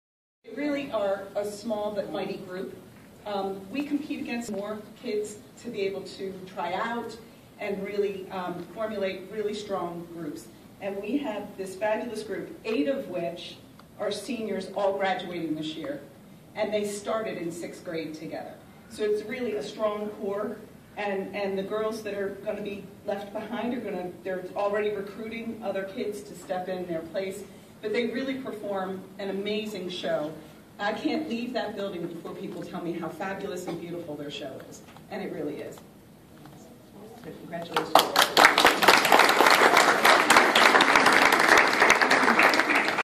The Radnor Township School Board commended the high school Color Guard fot winning "Season of Firsts
By: Linda Stein - Reporter at Main Line Media & author of the mystery novel Trenton Dead.